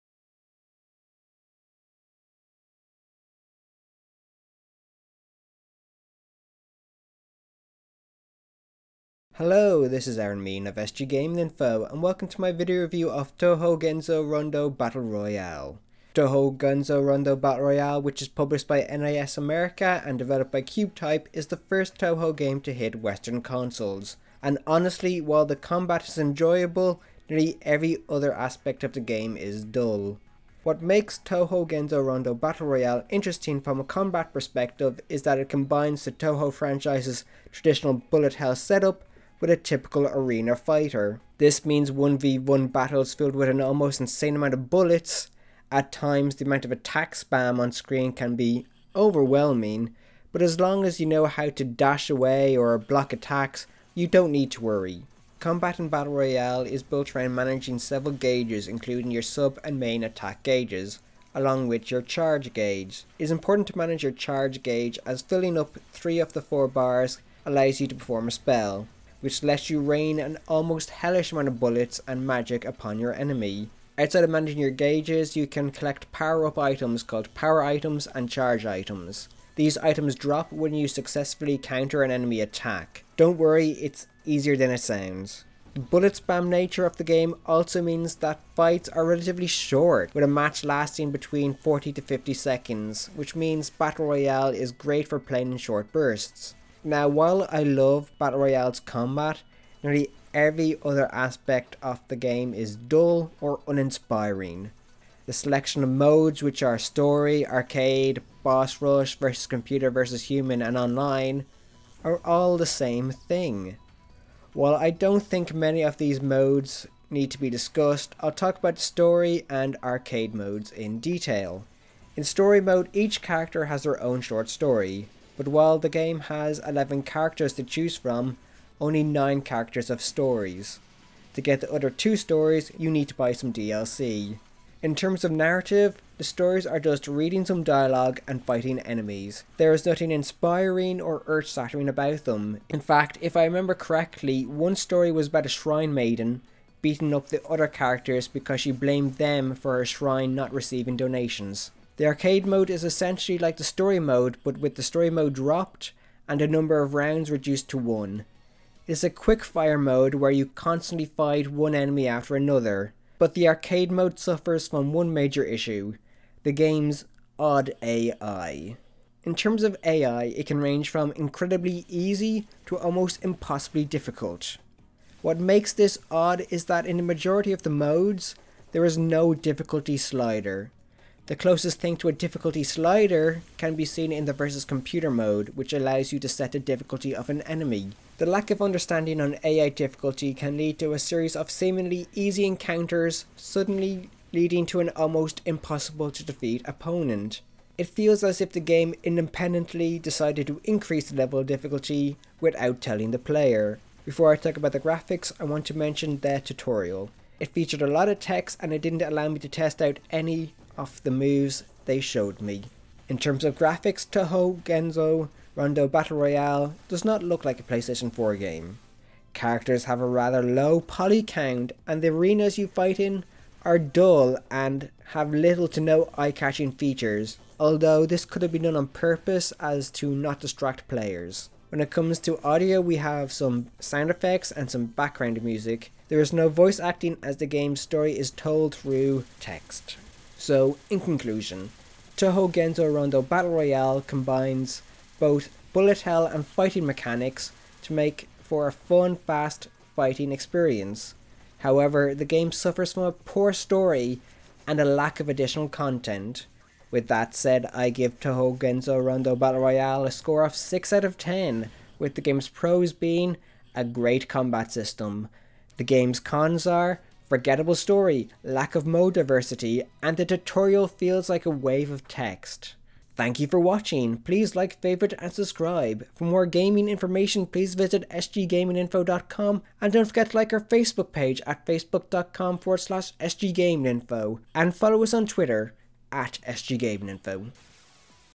Let's review - Touhou Genso Rondo Battle Royale
This is SG Gaming Info's video review of CUBETYPE's Touhou Genso Rondo Battle Royale.

A review copy was provided by the game's publisher.

If you wish to comment or provide feedback please use the comment section below. Please note that comments that are created to cause hatred, trolling or are generally unhelpful will be ignored.